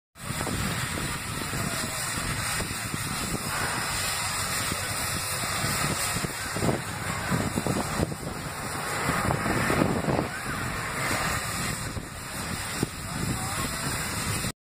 Mini Ha-Ha steam
Steam coming out of the Mini Ha-Ha while docked in Lake George, NY